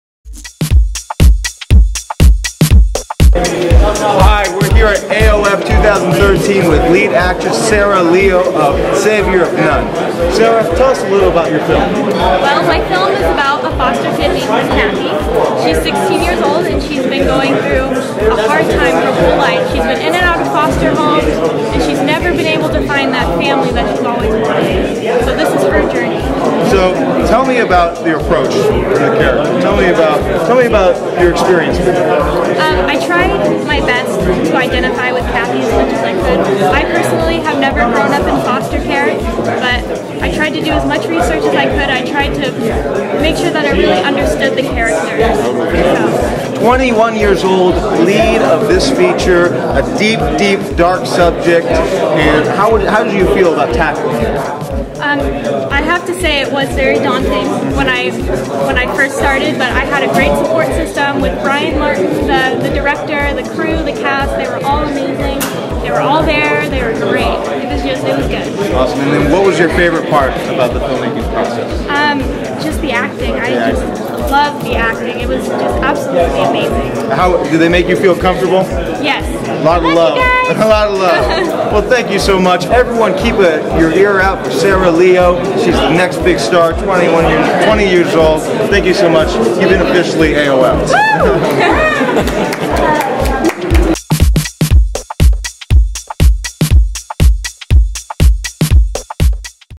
Sarah Leo at Action On Film Festival
Sarah Leo is the star of Savior Of None, a very intense and powerful film.  Here she talks about working with Bryan Martin and the rest of her cast to bring the film to live.